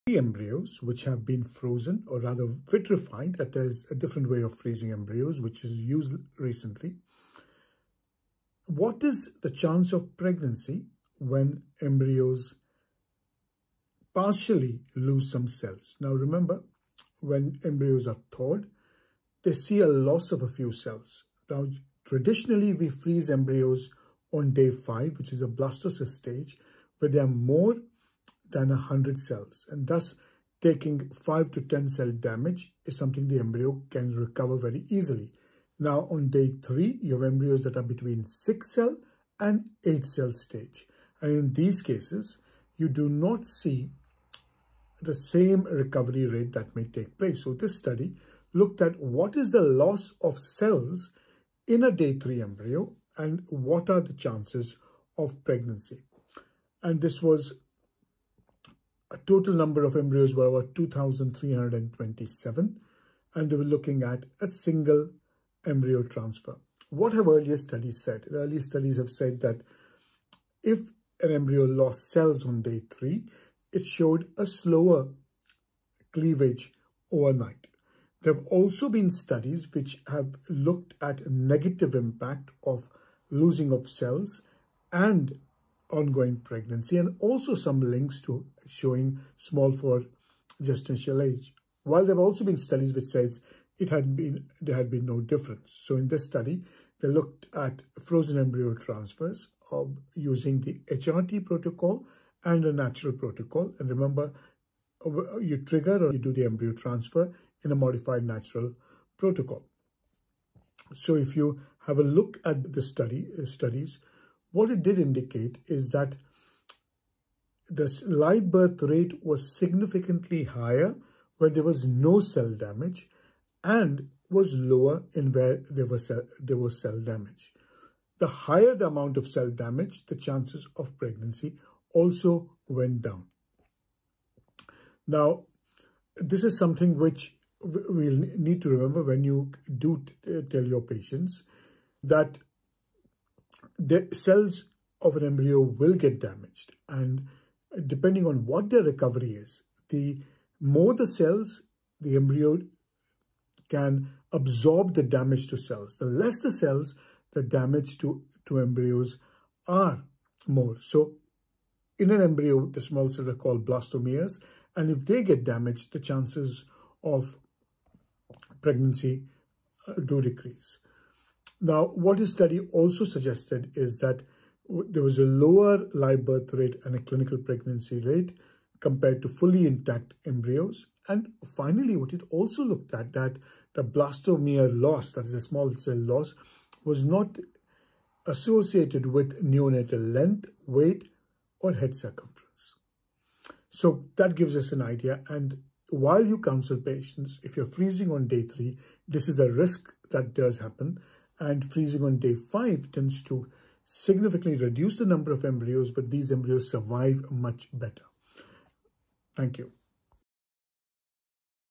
Freezing and Thawing of Day 3 embryos . How much cell damage affects results
on day 3 embryos have less cell - 6 to 8 cells. on thawing there is a risk of loosing come cells . Does that affect results